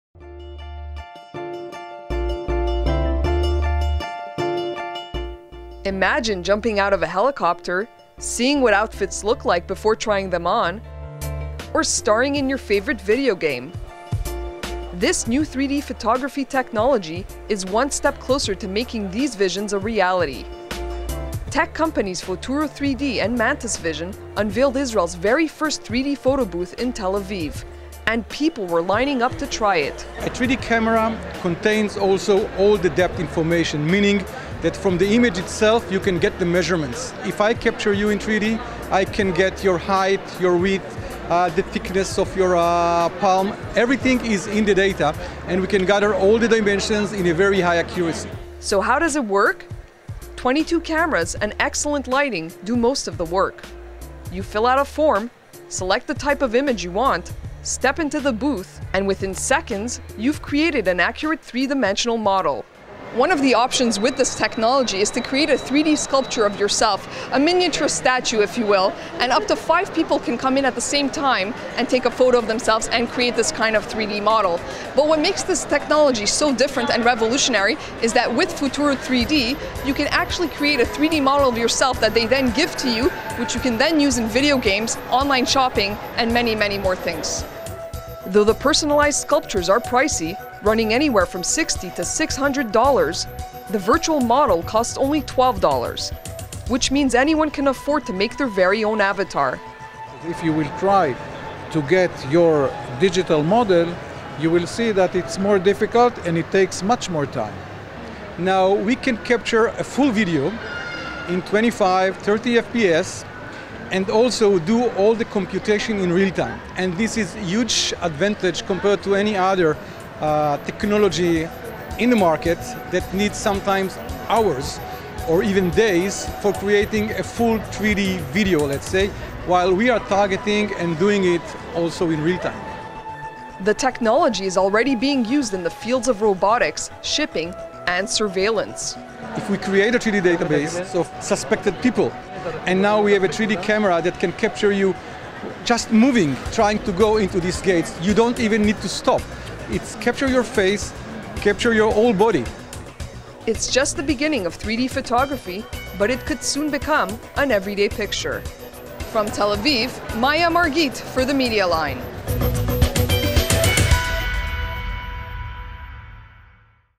New Israeli 3D Photography Technology Unveiled
Tech companies Photuro and Mantis Vision unveiled Israel's very first 3D photo booth in Tel Aviv and people were lining up to try it. The technology could be used for gaming, online shopping - and surveillance.